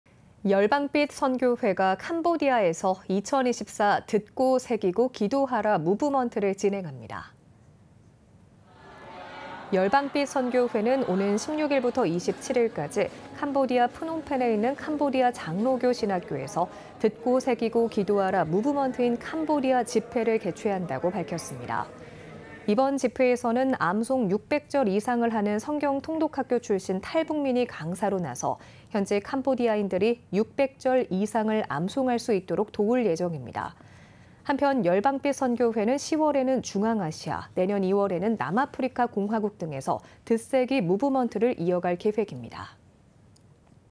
열방빛선교회, ‘2024 듣새기 무브먼트 인 캄보디아’ 진행 (이한승)ㅣCTS뉴스
열방빛선교회가 캄보디아에서 ‘2024 듣고 새기고 기도하라 무브먼트’를 진행합니다.

열방빛선교회는 오는 16일부터 27일까지 캄보디아 프놈펜에 있는 캄보디아장로교신학교에서 ‘듣고 새기고 기도하라 무브먼트 인 캄보디아 집회’를 개최한다고 밝혔습니다.

이번 집회에서는 암송 600절 이상을 하는 성경통독학교 출신 탈북민이 강사로 나서 현지 캄보디아인들이 600절 이상을 암송할 수 있도록 도울 예정입니다.

한편 열방빛선교회는 10월에 중앙아시아, 내년 2월에 남아프리카공화국 등에서 듣세기 무브먼트를 이어갈 계획입니다.